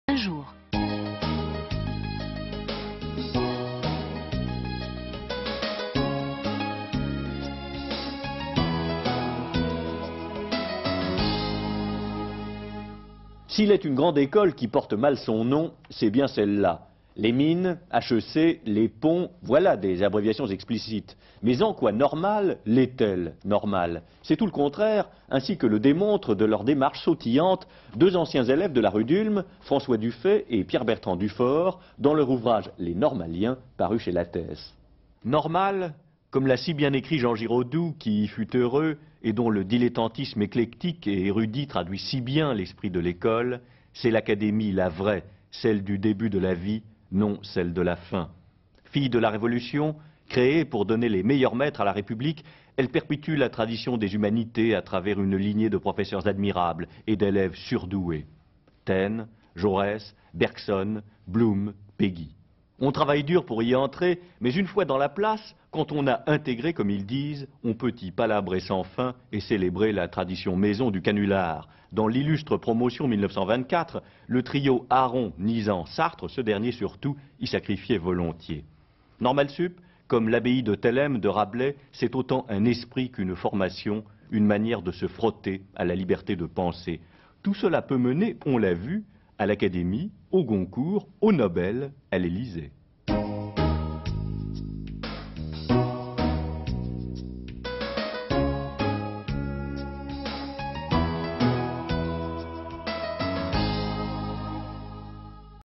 F. Dufay et P-B. Dufort : Les Normaliens
9 novembre 1993
Olivier BARROT présente "Les Normaliens", ouvrage de François DUFAY et Pierre-Bertrand DUFORT sur l'Ecole normale supérieure, ses professeurs admirables, ses élèves exeptionnels, son esprit, sa formation... Il cite particulièrement la promotion de 1924 avec ARON, NIZAN, SARTRE, entre autres. Images d'archive INA